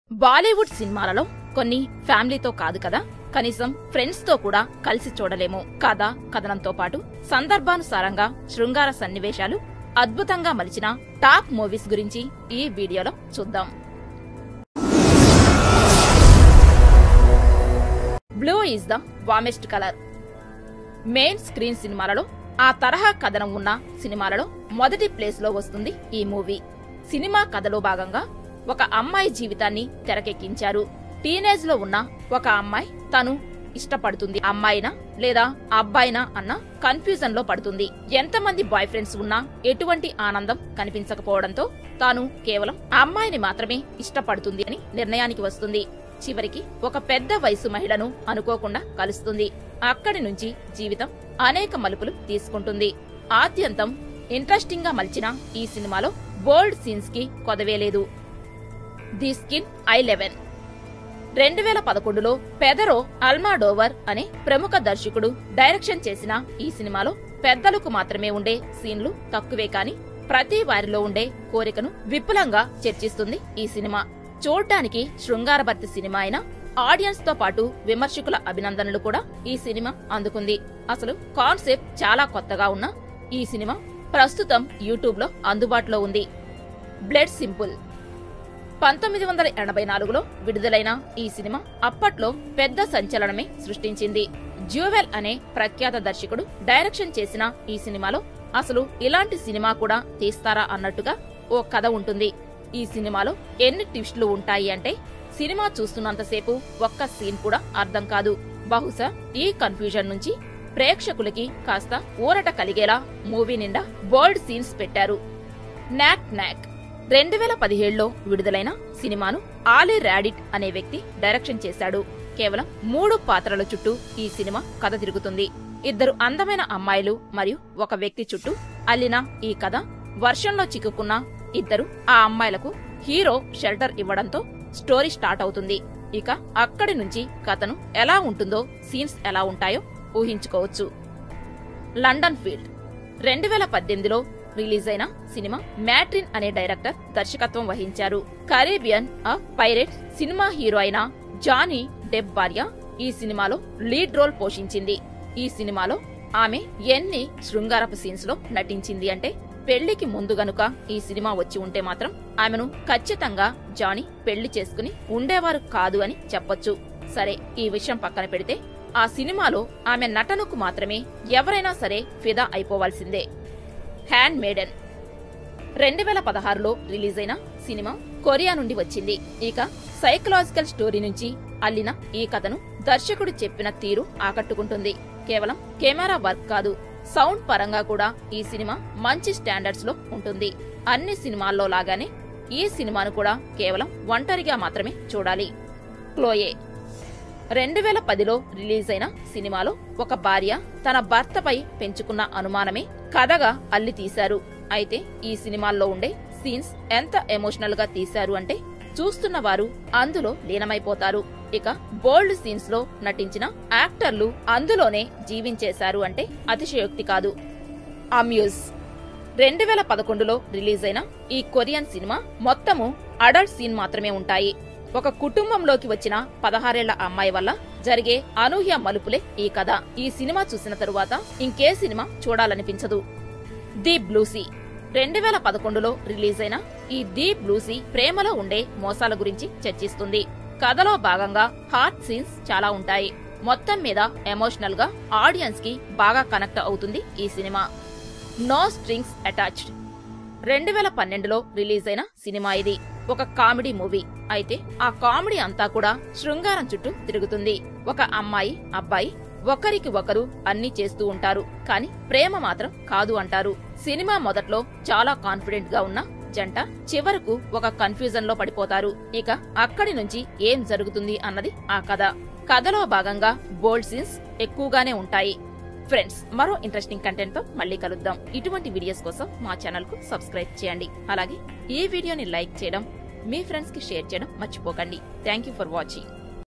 ఇవి చూసినా పోర్న్ చూసినా ఒకటే..ఈ సినిమాలను కనీసం ఫ్రెండ్స్ తో కూడా చూడలేము | Hidden Facts Telugu
ఈ 10 సినిమాలను కనీసం ఫ్రెండ్స్ తో కూడా చూడలేము...టాప్ బాలీవుడ్ బోల్డ్ మూవీస్ | Interesting Facts In Telugu | Hidden Facts Telugu |